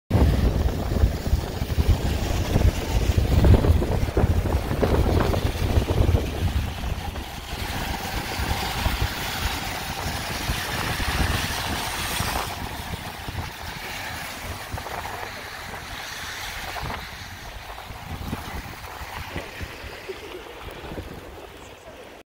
Ballachulish Scotland United Kingdom Loch Achtriochtan
Driving along Loch Achtriochtan on a rainy day. 3/8/2020